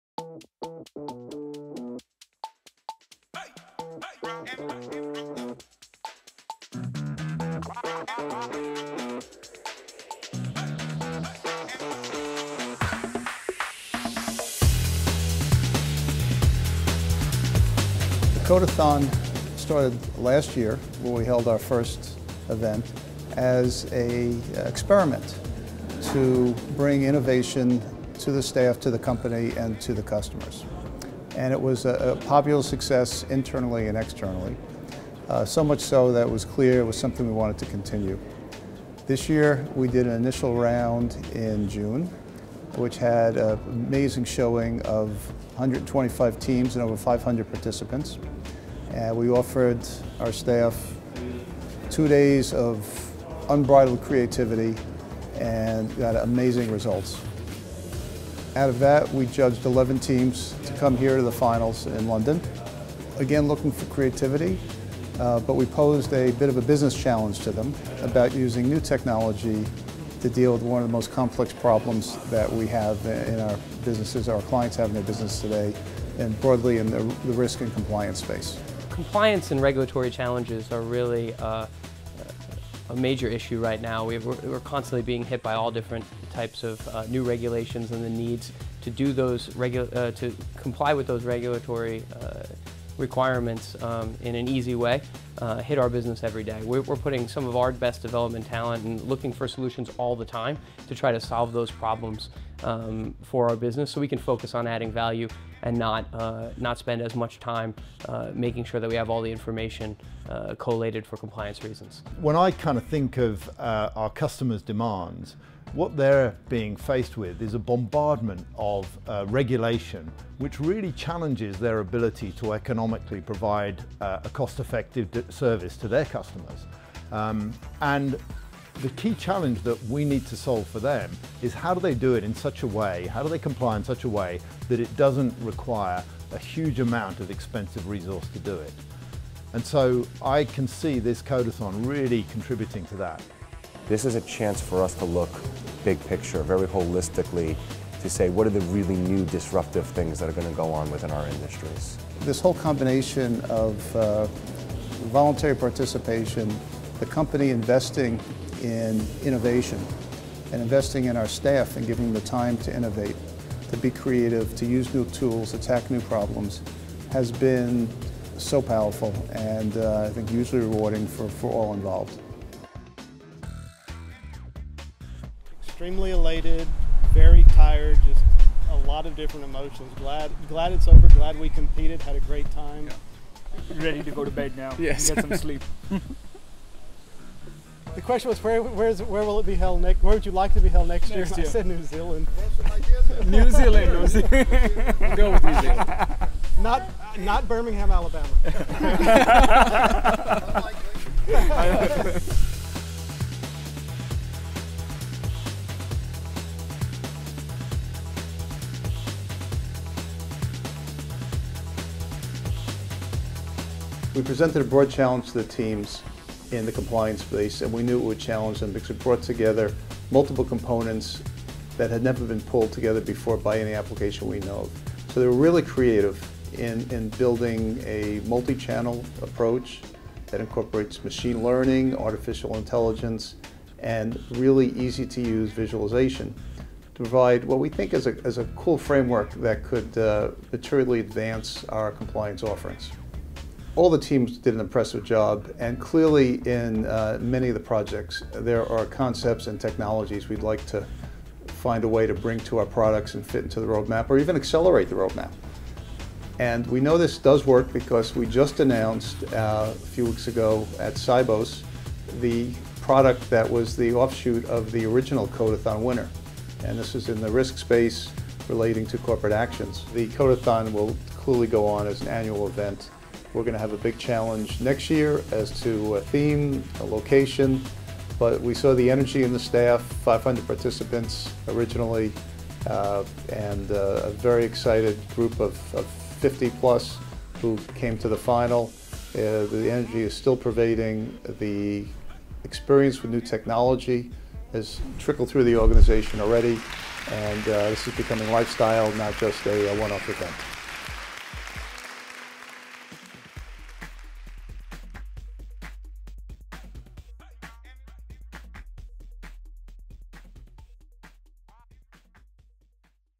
SunGard Global Coder Cup 2014: The Final
Eleven teams from seven countries met at London’s Emirates Stadium to participate in the final championship round of SunGard’s second annual codeathon. The finalists were given 48 hours and a clear mission: to harness big data technology to create new compliance solutions for financial services firms.